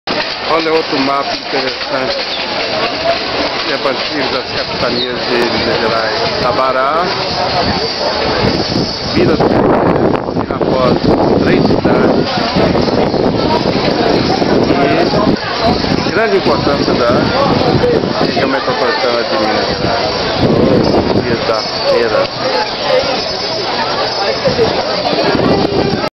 Guilherme Jabur mostra um Mapa antigo da Capitania das Minas Gerais parte um
ESSES  MAPAS DA CAPITANIA DE MINAS GERAIS, JÁ OBSERVAVA A COMARCA E CIDADES DE VILA RICA, CAETÉ, SABARÁ, RAPOSOS. UM MAPA AO QUAL ESTAVA FIXADO TEMPORARIAMENTE NO PALACIO DAS ARTES NA AVENIDA AFONSO PENA PERTO DO PARQUE MUNICIPAL E ERA UM DOMINGO DIA DE FEIRA DE ARTESANATO AQUI EM BELO HORIZONTE